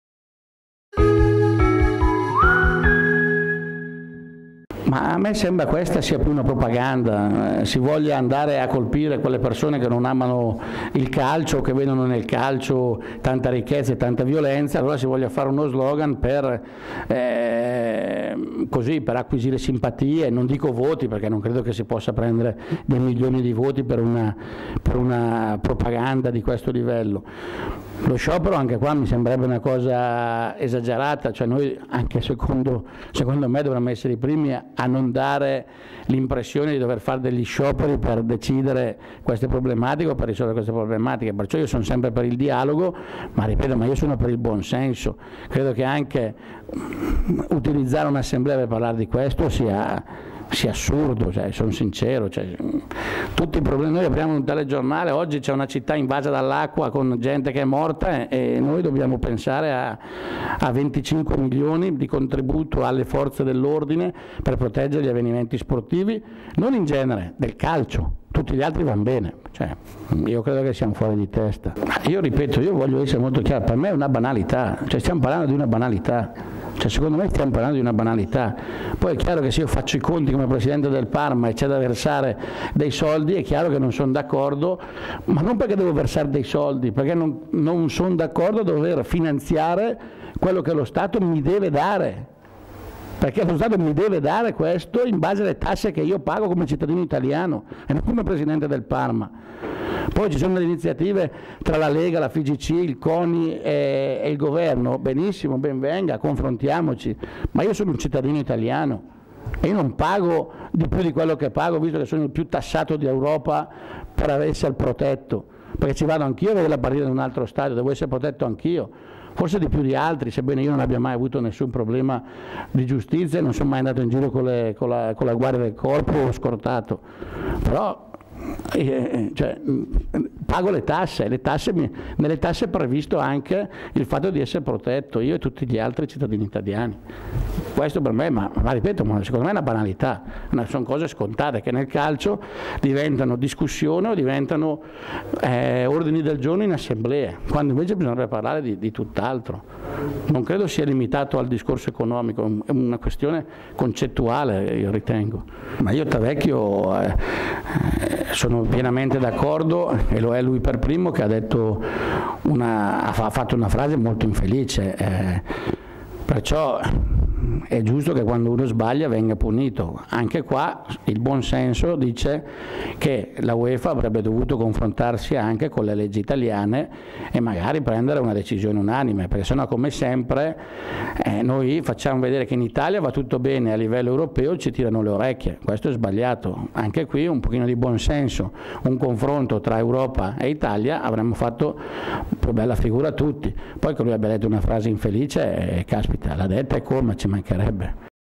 Ghirardi: "La proposta di Renzi? Propaganda. Tavecchio è giusto che paghi!"
Il presidente del Parma, intervenuto in Lega Calcio, ha bocciato l'idea rilanciata dal Presidente del Consiglio che, nei giorni scorsi, aveva chiesto un contributo economico alle società di calcio, da destinare alle forze dell'ordine impegnate negli stadi.